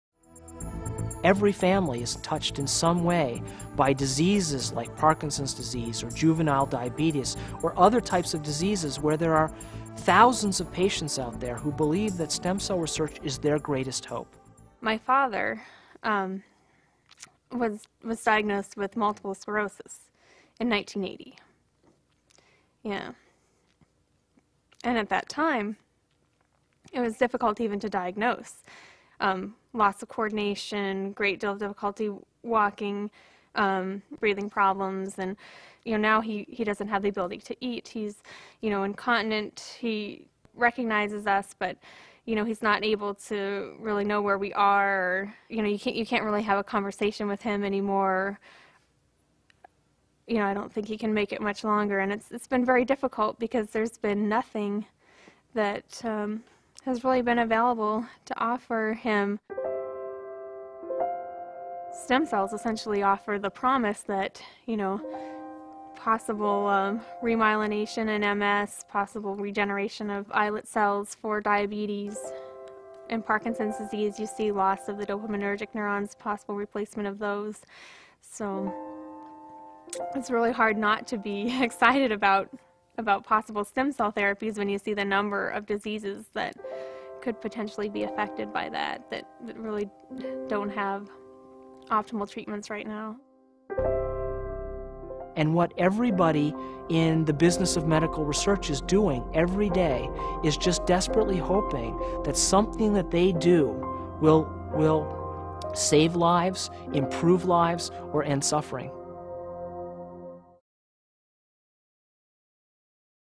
The Importance of Stem Cells
Morrison and graduate student Eve Kruger discuss importance of stem cells